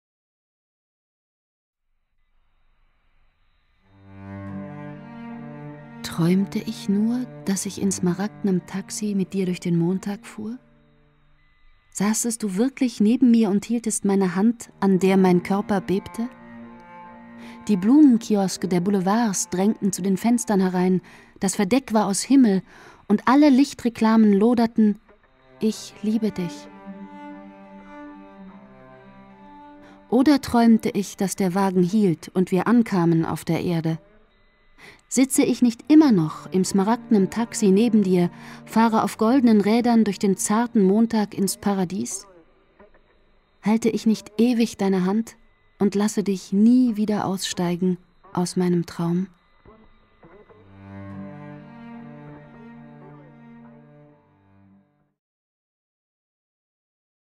CLAIRE GOLL - TRÄUMTE ICH NUR
Dichtung von Claire Goll /
Rezitation: Ulrike Grote (Foto) /
Anmerkung: Sowie Gott in Paris eine schöne Frau entstehen lässt, schickt der Teufel als Antwort einen Narren, der sie aushält. (C.Goll)

Träumte ich nur,
Daß ich in goldenem Taxi
Mit dir durch den Montag fuhr?
Saßest du wirklich neben mir
Und hieltest meine Hand
An der mein Körper bebte?
Die Blumenkioske der Boulevards
Drängten zu den Fenstern herein,
Das Verdeck war aus Himmel
Und alle Lichtreklamen loderten:

Oder träumte ich,
Daß der Wagen hielt
Und wir ankamen auf der Erde?
Sitze ich nicht immer noch
In goldenem Taxi neben dir
Fahre durch den zarten Montag
Ins Paradies?
Halte ich nicht ewig deine Hand
Und lasse dich nie wieder aussteigen
Aus meinem Traum?